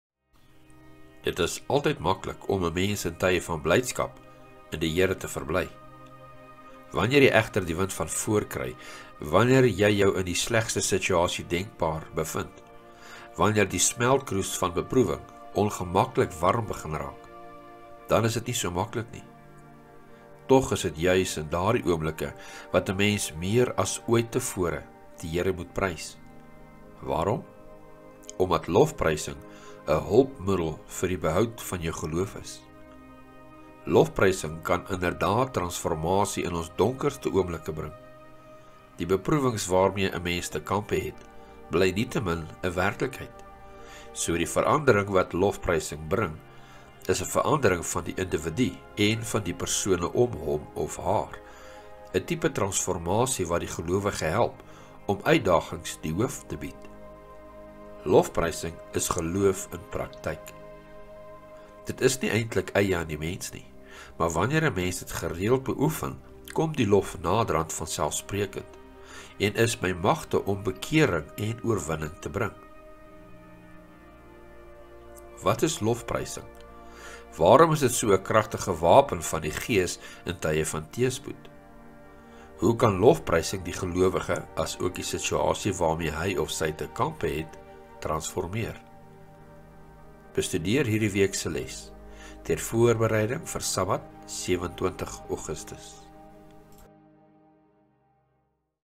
Sabbatmiddag 20 Aug
Les 9 – ’n Lewe van lofprysing
20 tot 26 Augustus 2022.
Derde Kwartaal 2022 – Saam met Christus in die smeltkroes

Aanbieder:
David Kühn
CREDIT: